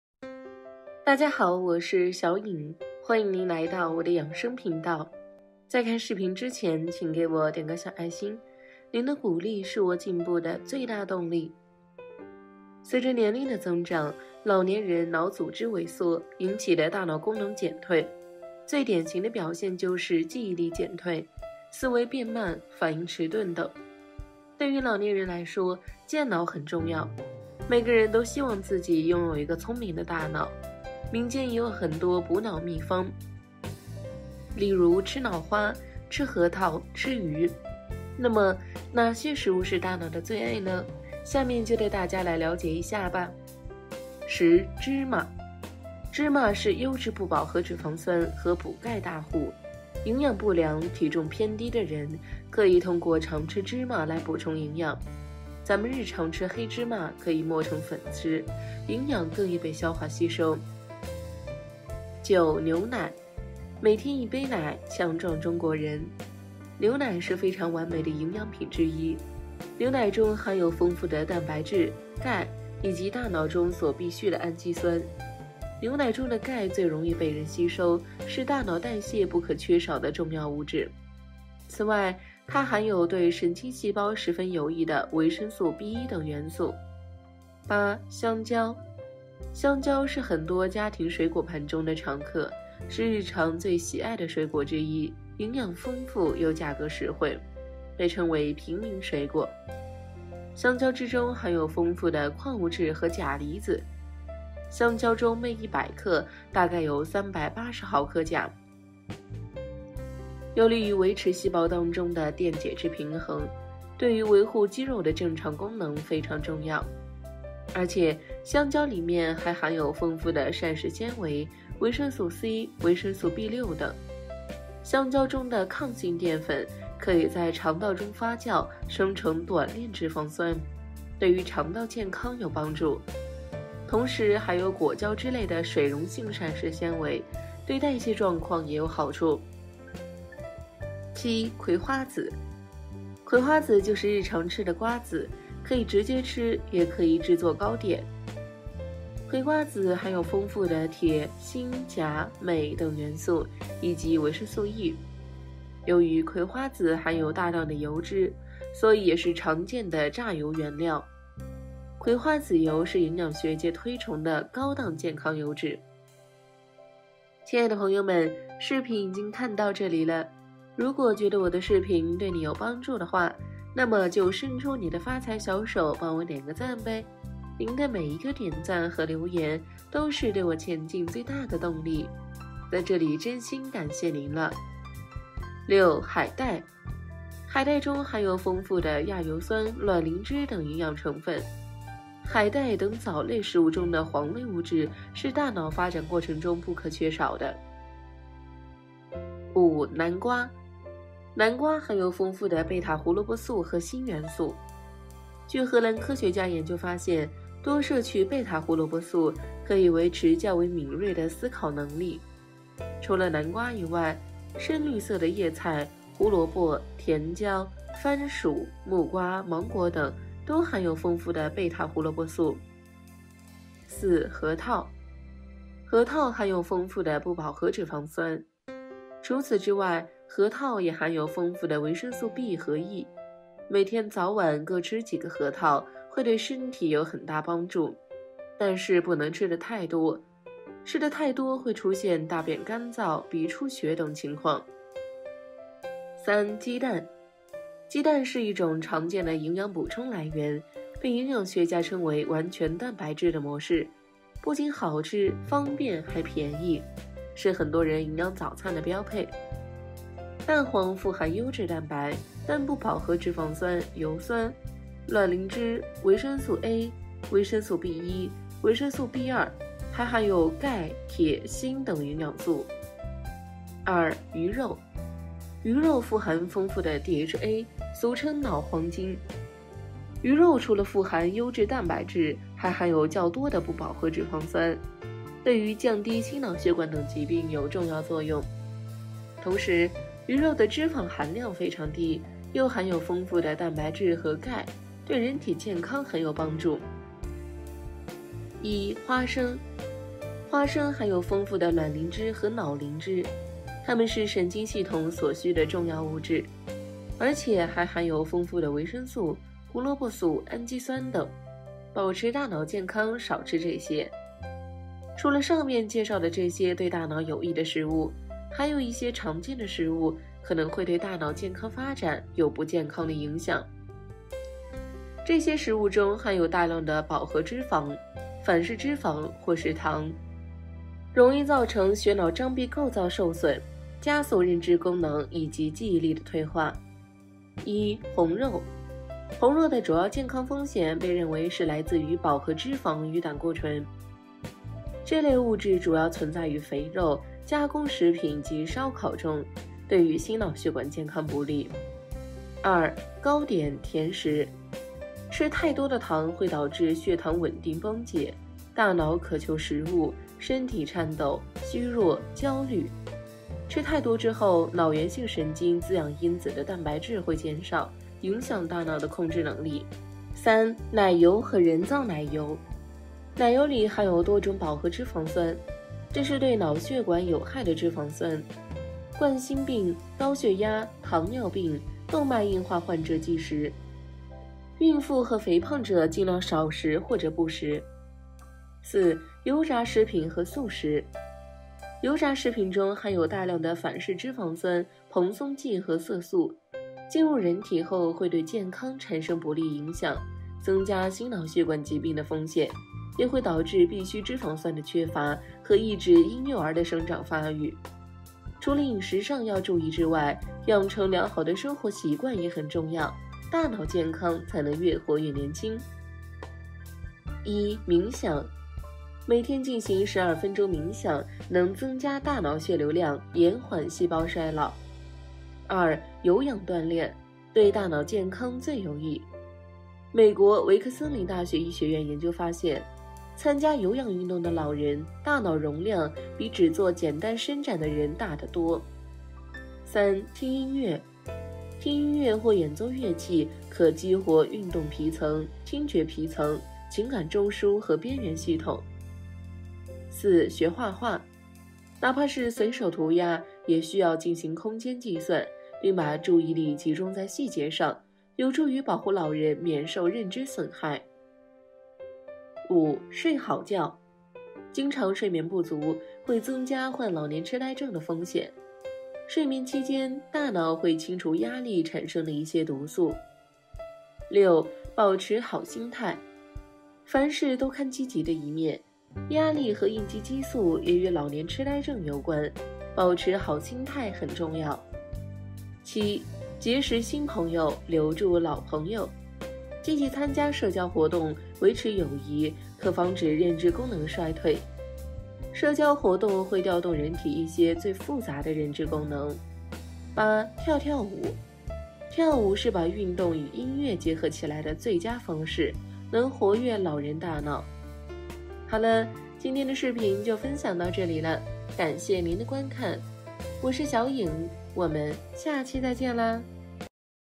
大腦最愛的10種食物，核桃只排第4，第1誰也想不到！防血栓，防癡呆，還能防癌，吃到就是賺到【小穎養生】，防血栓食物，防癡呆食物，防癌食物
大腦最愛的10種食物，核桃只排第4，第1誰也想不到！防血栓，防癡呆，還能防癌，吃到就是賺到【小穎養生】，

多謝你支持 有空給我一個Like 🙏